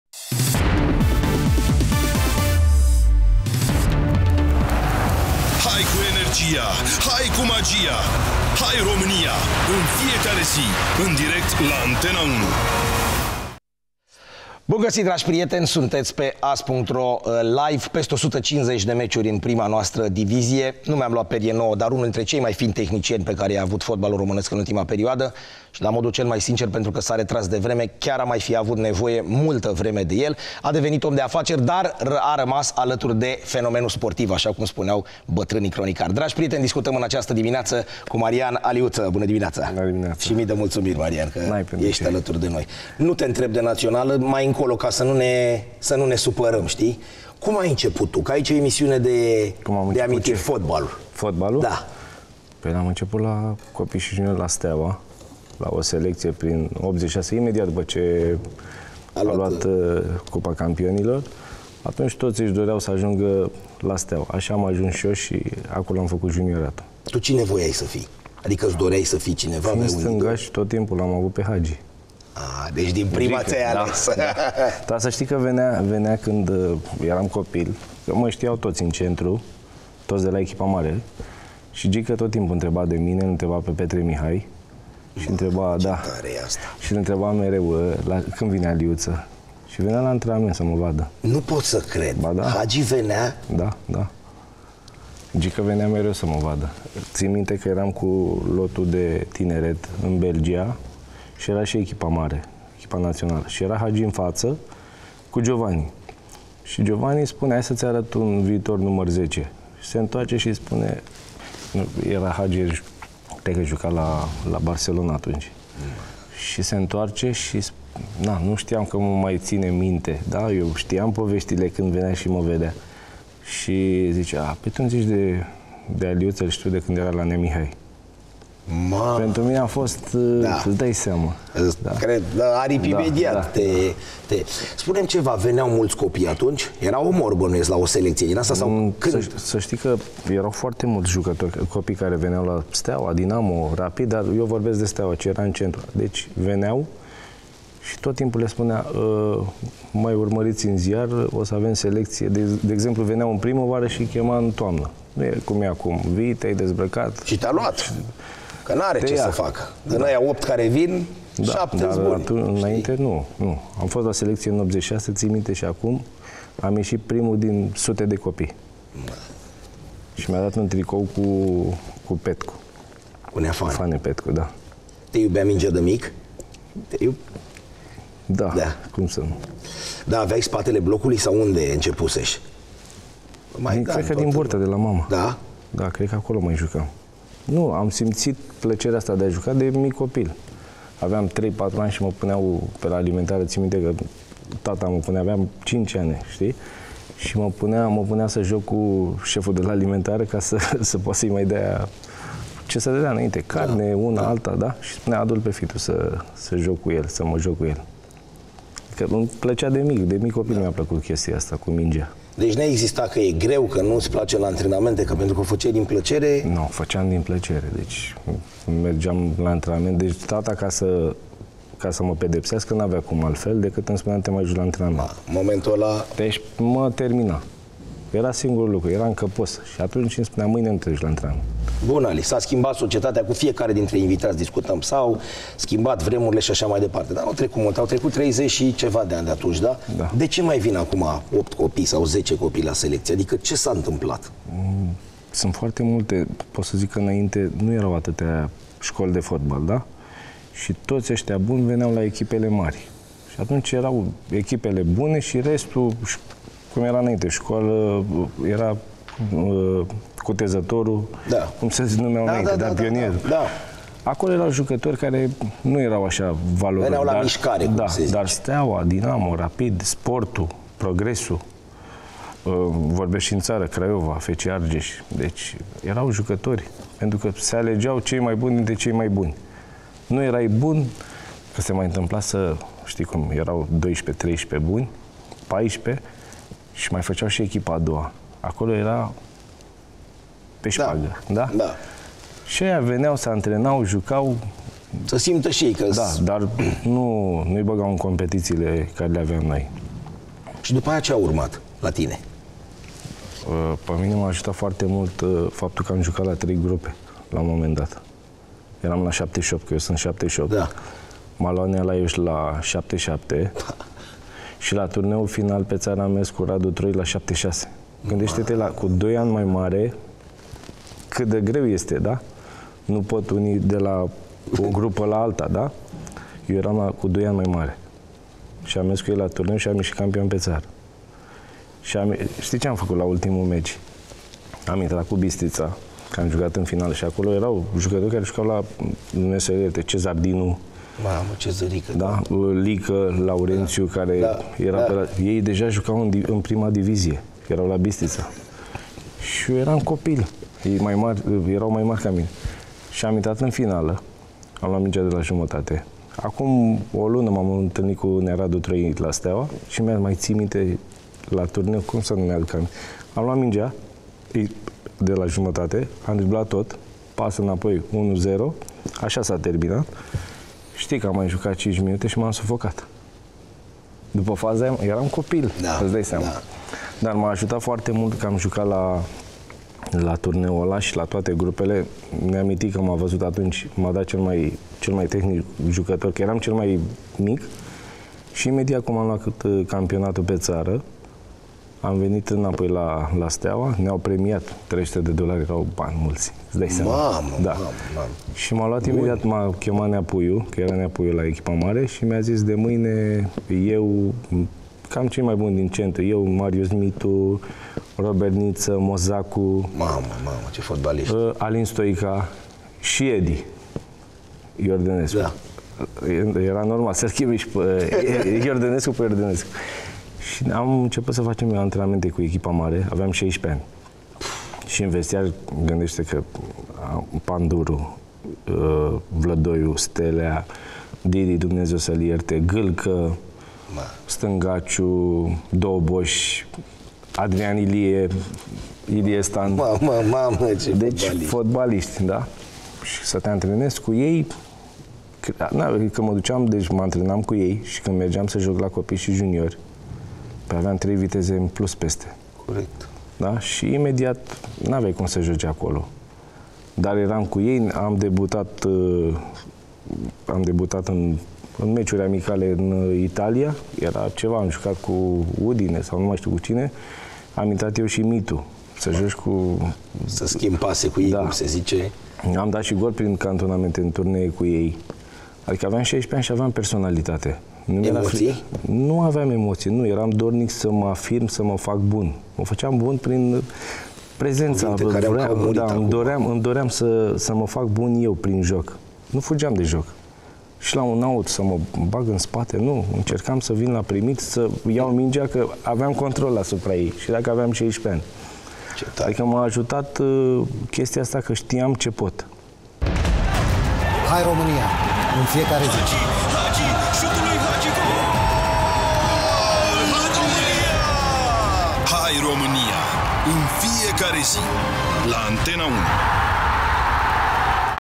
Marian Aliuţă la AS.ro LIVE, partea 1. Cât de mult l-a apreciat Gică Hagi când era junior
Marian Aliuţă a câştigat trei trofee în carieră: un titlu cu Steaua şi două Cupe cu Şahtior. Chindia (1996), Gloria (1997), Farul (1997 – 1998), Sheriff Tiraspol (1998 – 1999), Șahtior Donetk (2000 – 2002) şi Steaua (2002 – 2004) sunt echipele pentru care a jucat acesta de-a lungul carierei.